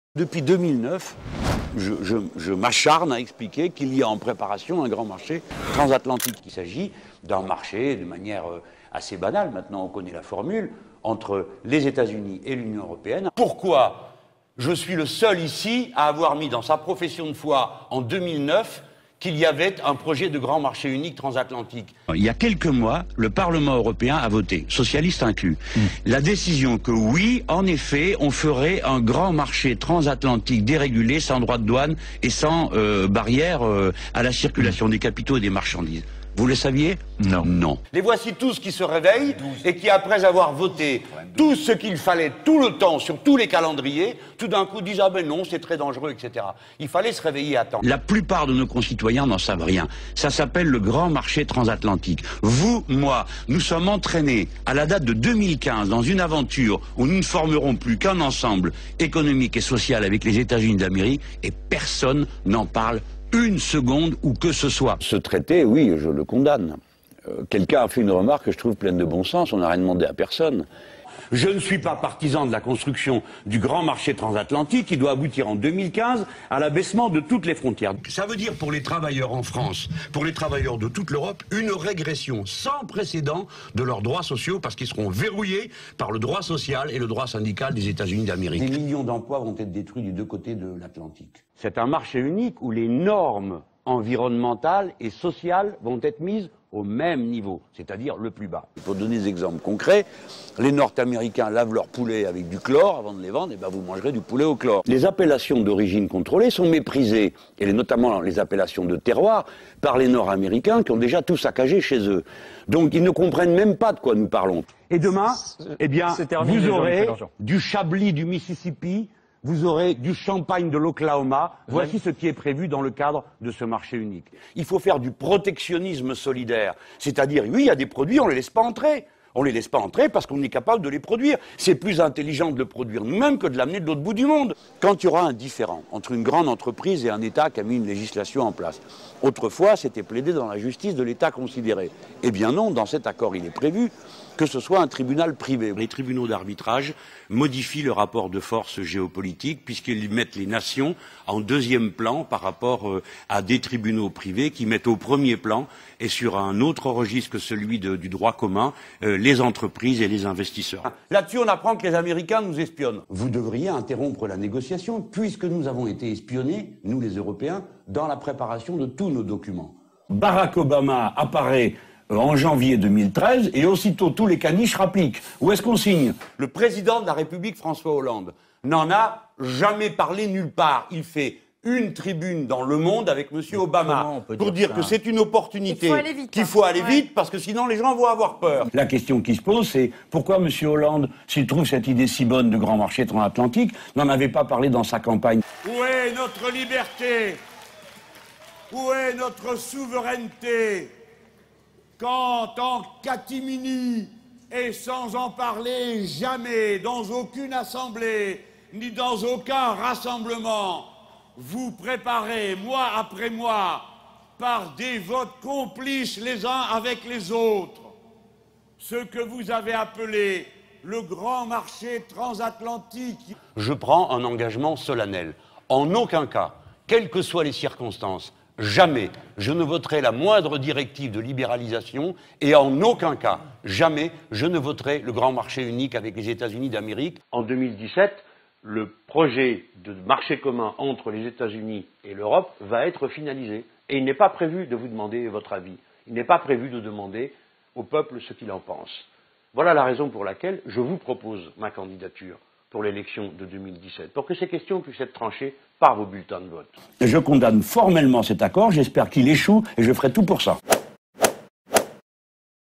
We Say NO to TAFTA! (with subtitles)
Since 2009, Jean-Luc Mélenchon is opposed to the Great Transatlantic Market project between Europe and the USA.